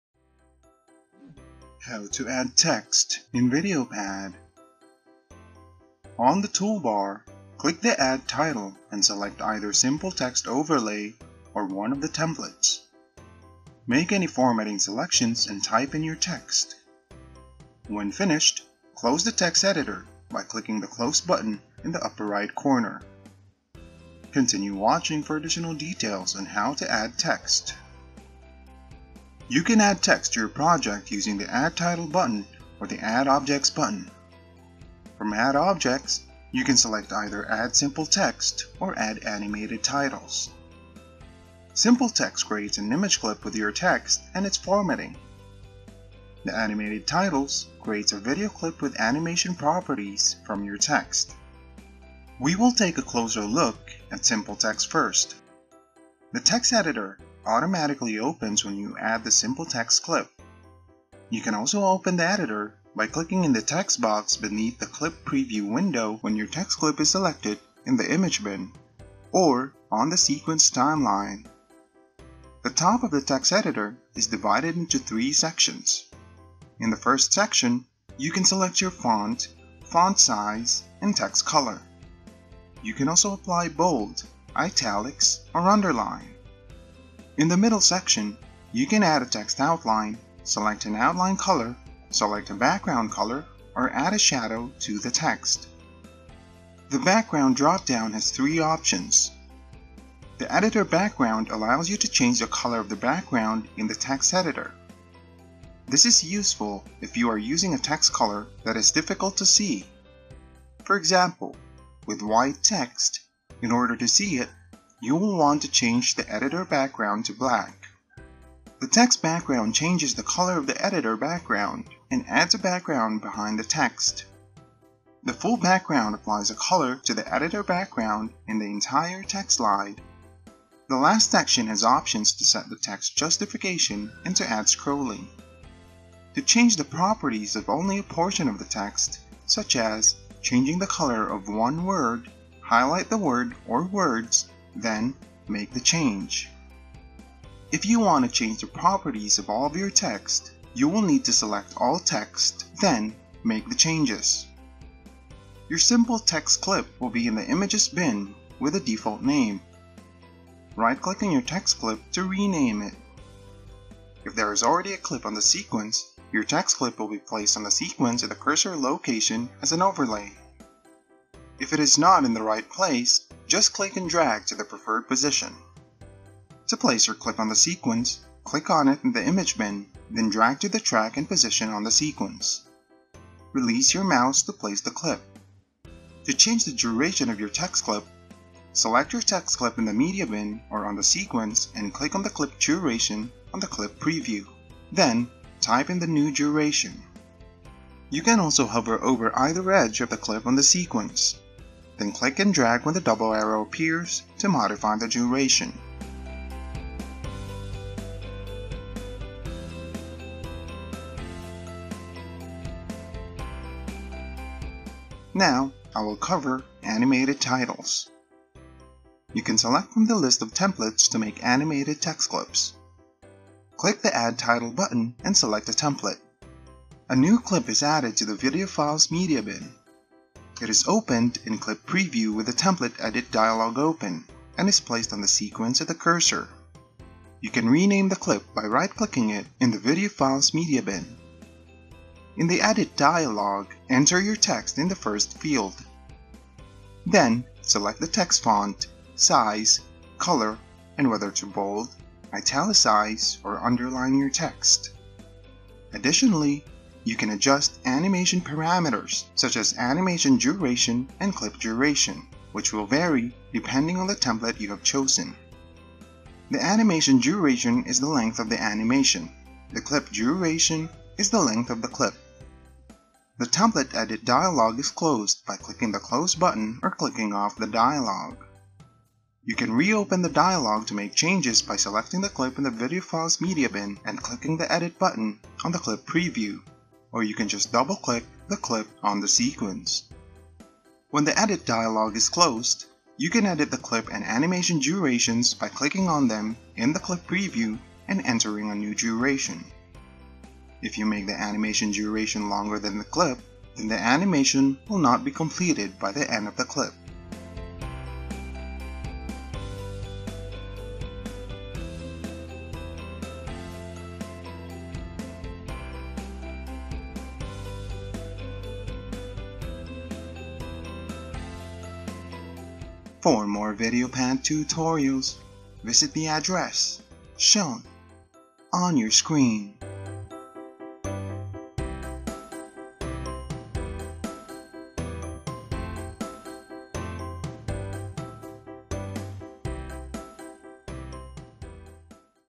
How to Add Text | VideoPad Video Editing Tutorial
This video will teach you how to add text and text overlays to add captions, title screens, credits and more to your videos with VideoPad Video Editor.

On the toolbar, click the Add Title and select either Simple Text overlay or one of the templates. Make any formatting selections and type in your text.  When finished, close the text editor by clicking the close button in the upper right corner.